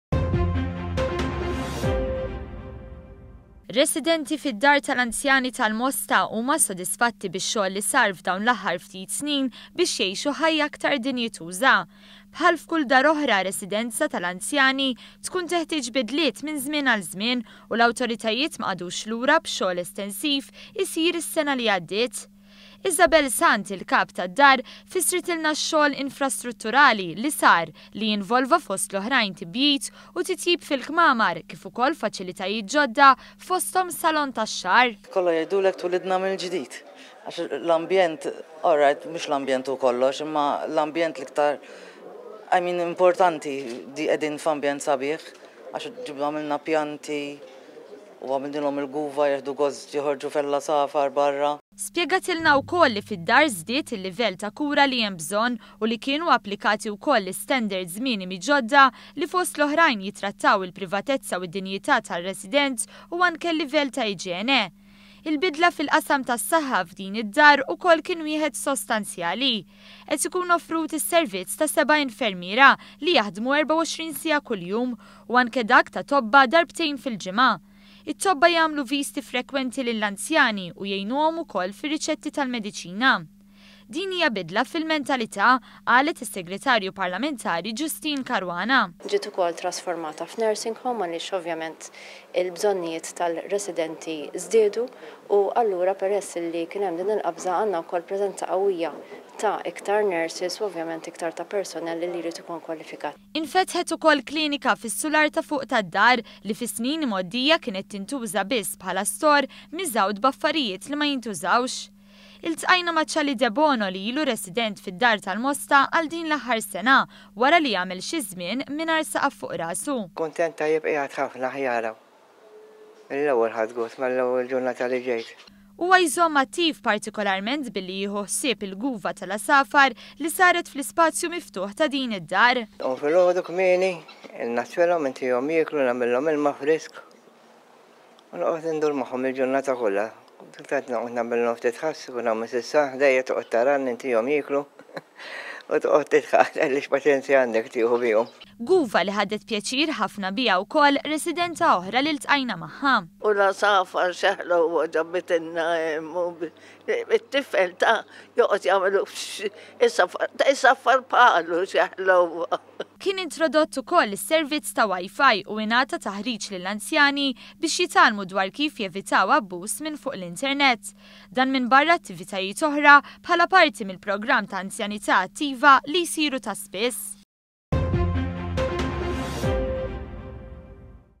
SODISFAZZJON FOST L-ANZJANI FID-DAR TAL-MOSTA.
ONE.COM.MT - Fl-aħħar sena, sar xogħol estensiv fid-dar tal-anzjani tal-Mosta,b’enfasi kbira saret fuq il-kwalita’ ta’ servizz li jingħata.